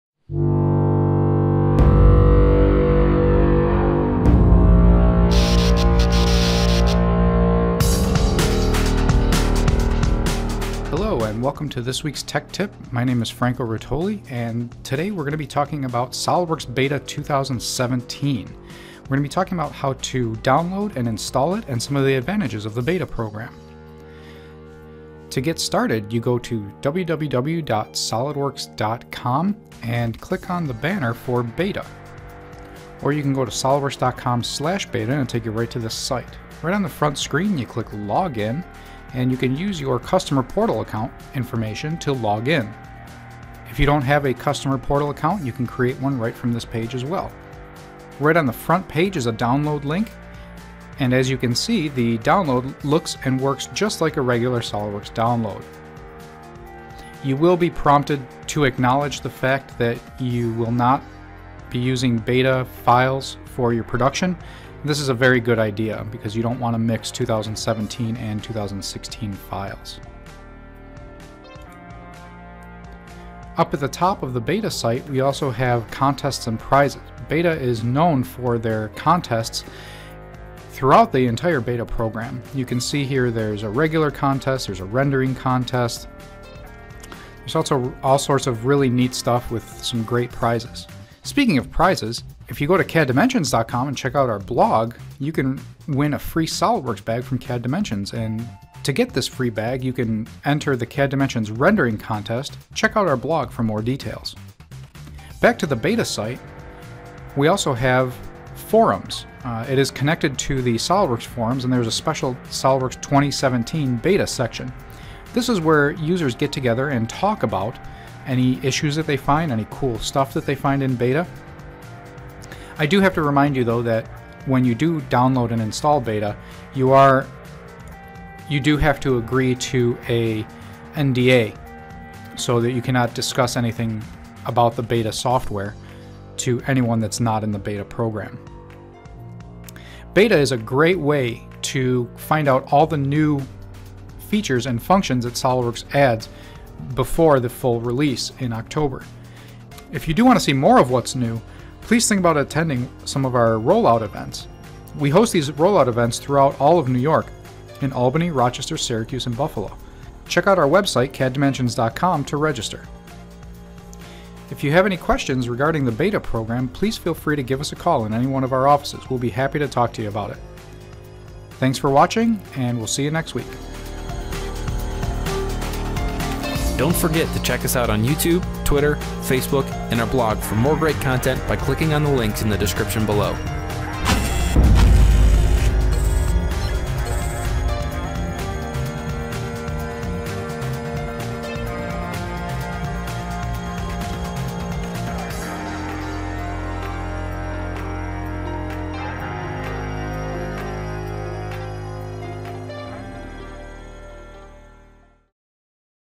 Tech Tip - SOLIDWORKS 2017 Beta Program
During this 3 minute Tech Tip, we are going to be talking about SOLIDWORKS Beta 2017. We will discuss how to download it, install it, and the advantages of the beta program.